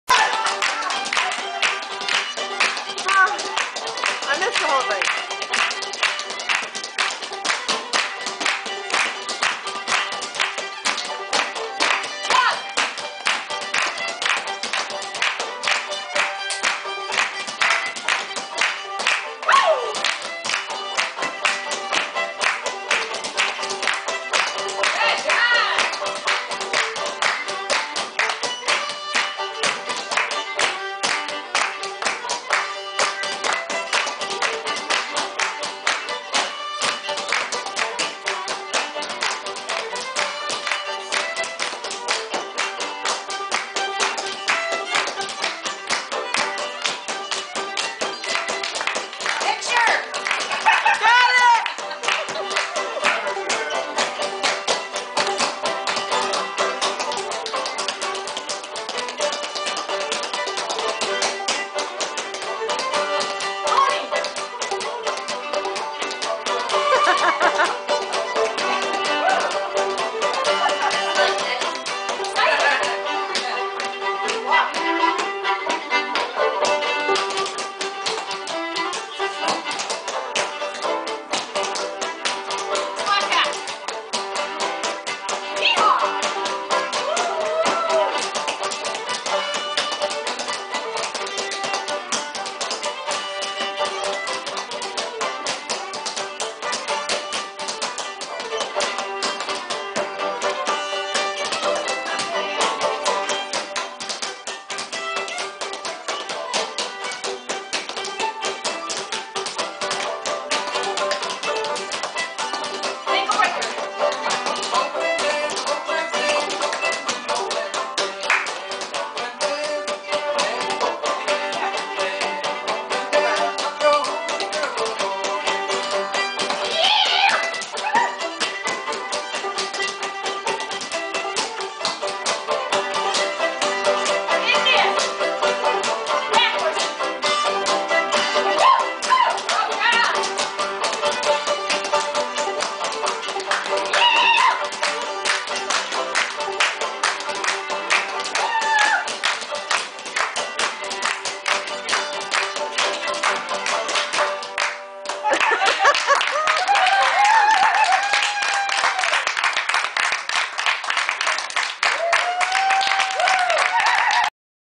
High-Low-Jack with Ann Whiting at Grand Isle House Concerts
Ann Whiting free style clogging with High-Low-Jack!